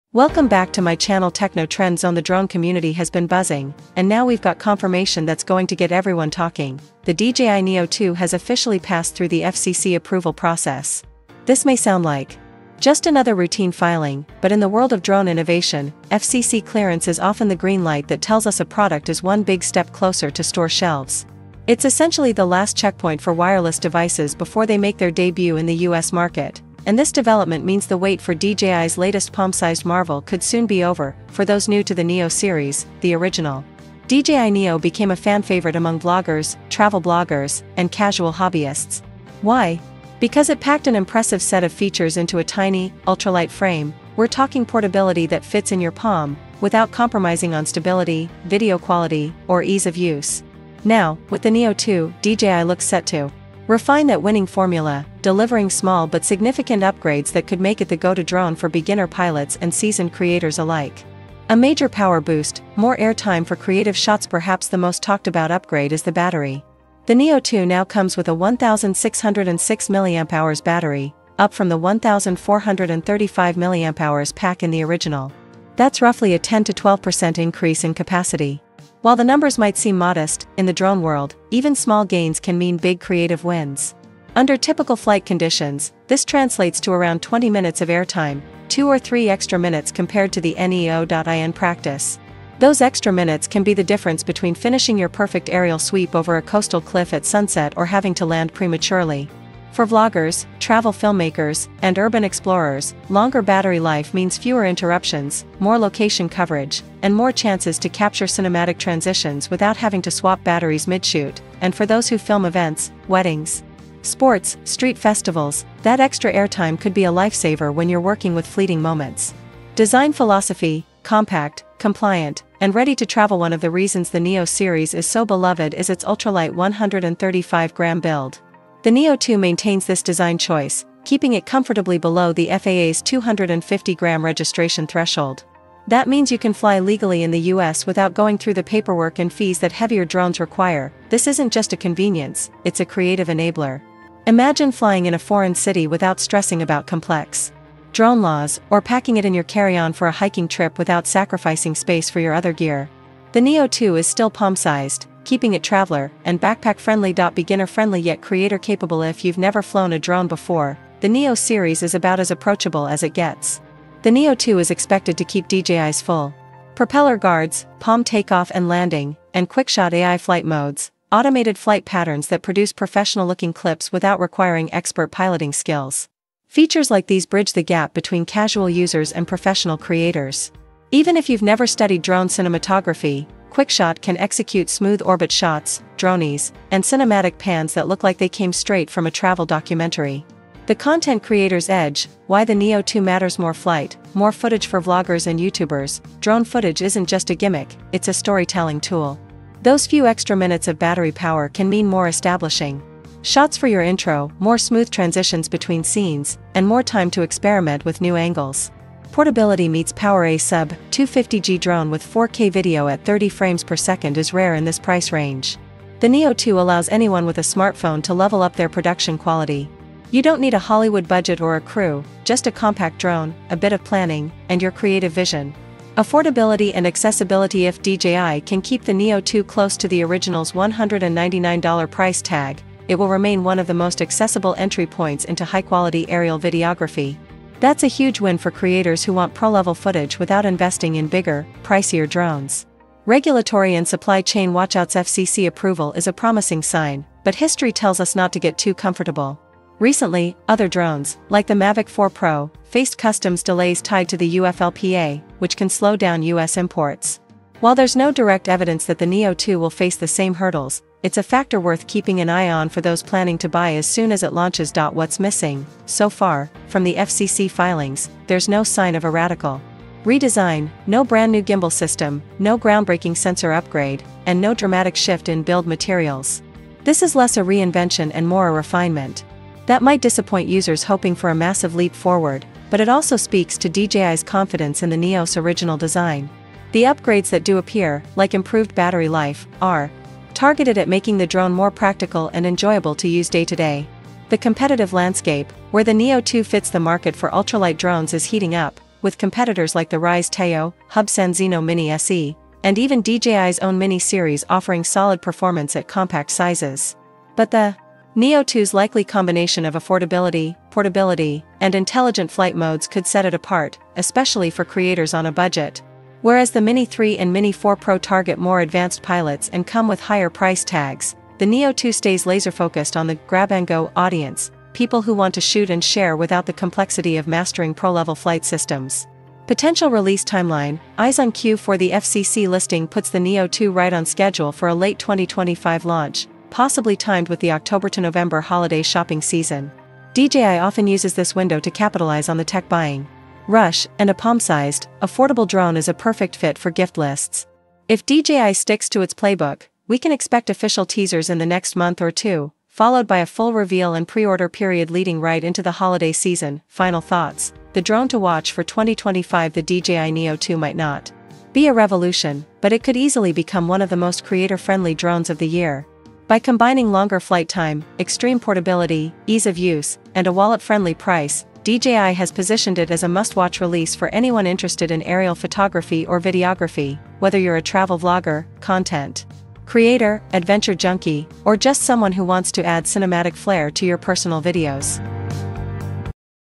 DJI Neo 2 Finally Confirmed: Compact Yet Much More Powerful!
The DJI Neo 2 is finally here, and it’s everything drone enthusiasts have been waiting for! This compact yet incredibly powerful drone packs advanced features, improved flight performance, and stunning camera quality into a sleek, portable design. Perfect for travelers, content creators, and professionals alike, the DJI Neo 2 sets a new standard in drone technology. In this video, we break down all the confirmed specs, features, release details, and why the Neo 2 is the perfect upgrade.